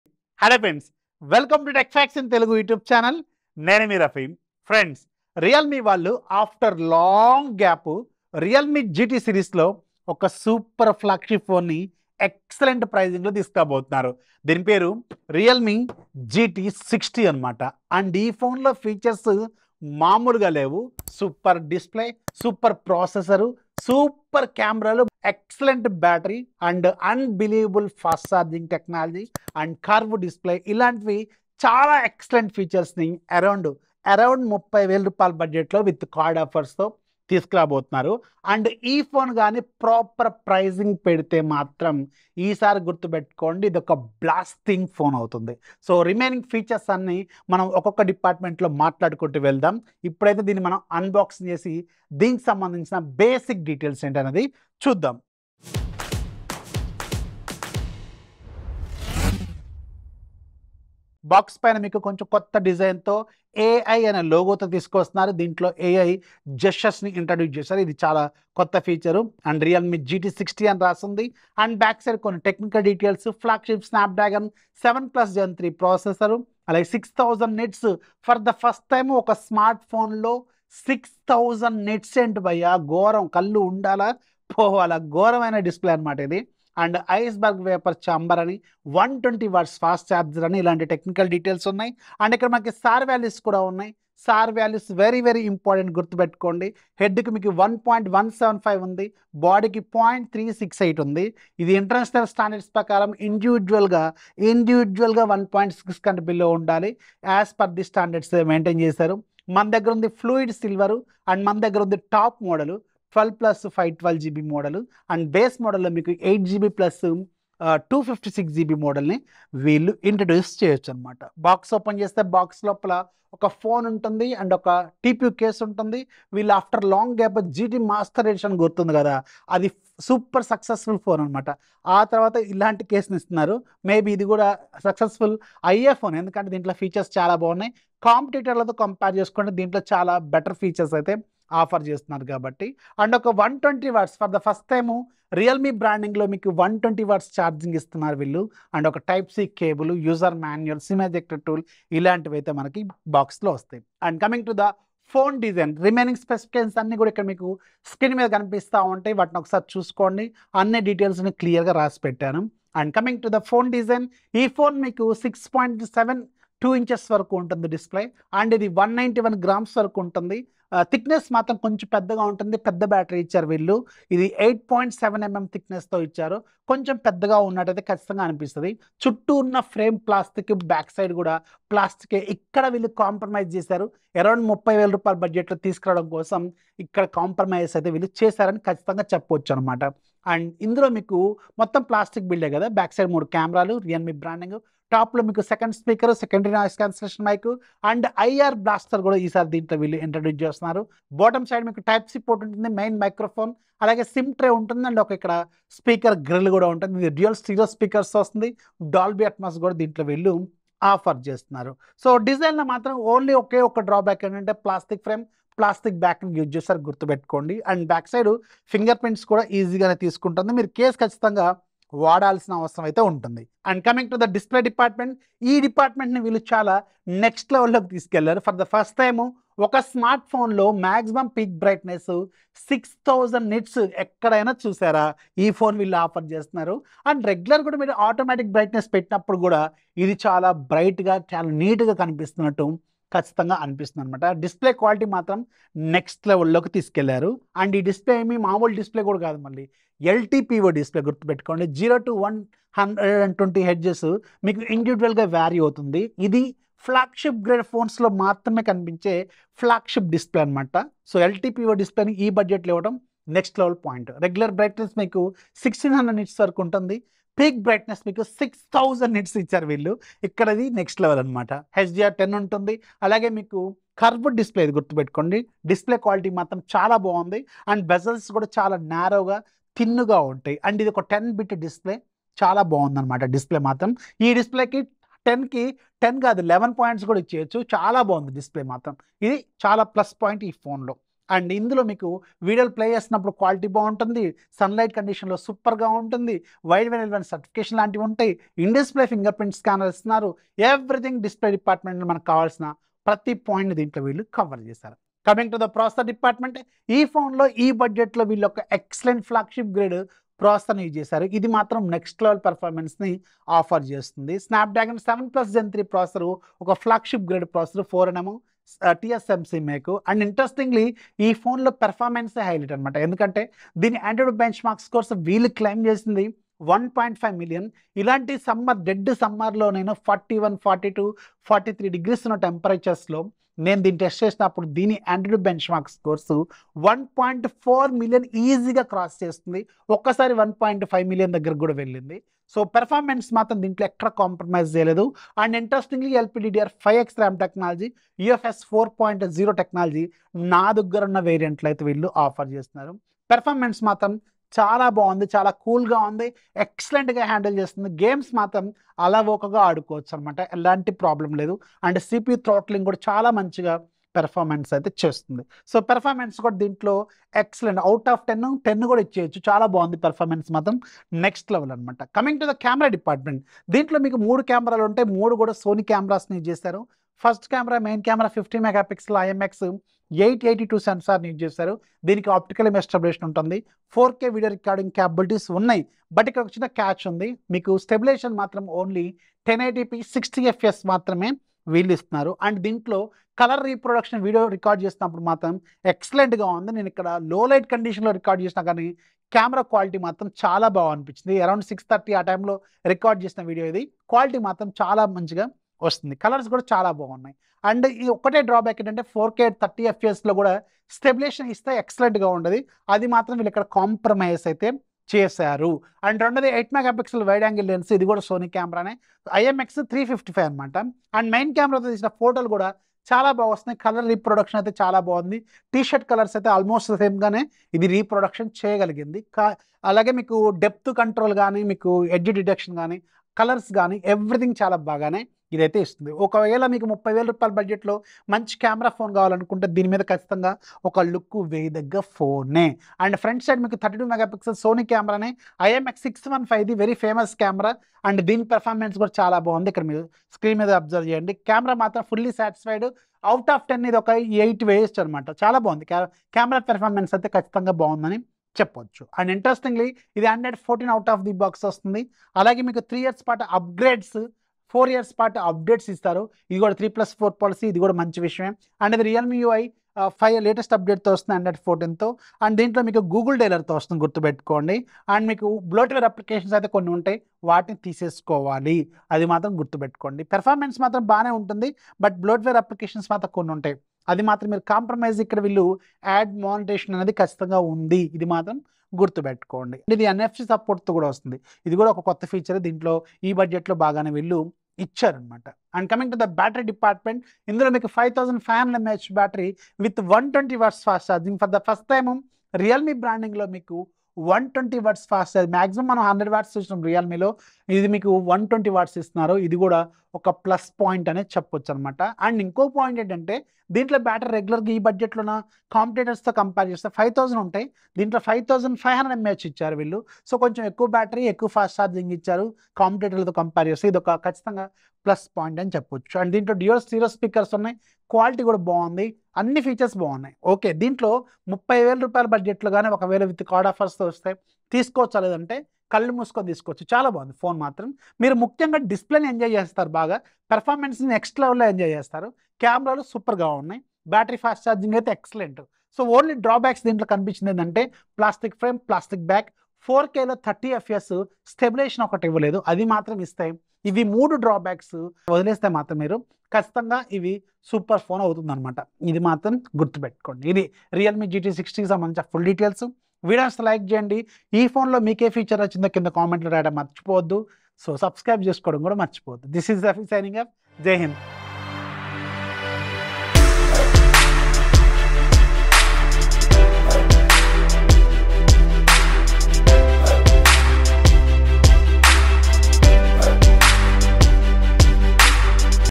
Realme GT 6T Unboxing ⚡Flagship at 24999 ⚡ Unbelivable Price ⚡
Hai Friends..

In this video i will share my quick review of realme GT 6T,

❤️❤️ With Love..TechFactsRafee ❤️❤️